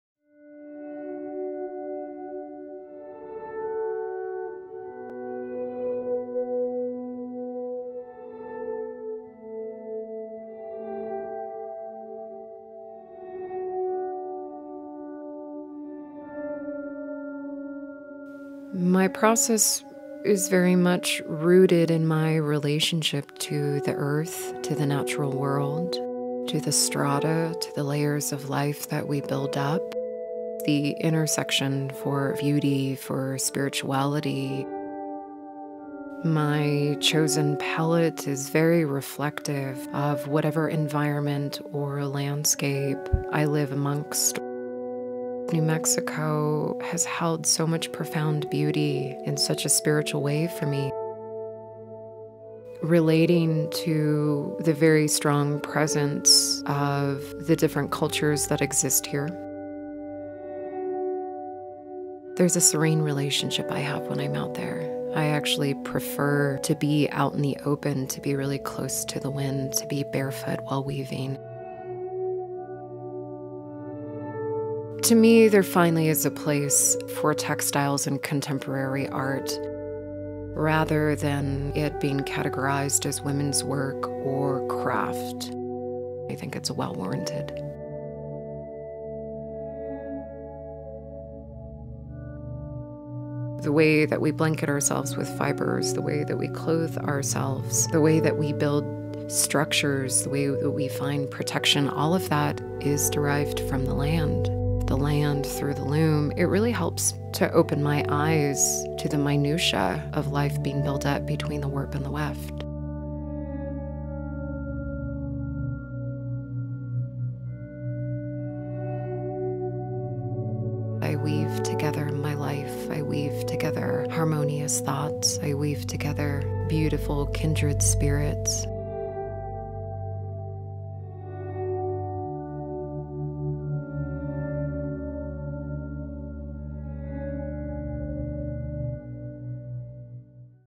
Woven Landscapes: The Work of Rhiannon Griego
Weaving together colors and textures drawn from the landscapes surrounding her, artist Rhiannon Griego is creating a beautiful new space for textiles in the art world. Her work captures a deep connection to self and environment, pulling on threads of the past to weave a brilliant representation of the present moment.

Discover more of Rhiannon’s work: @rhiannonmgriego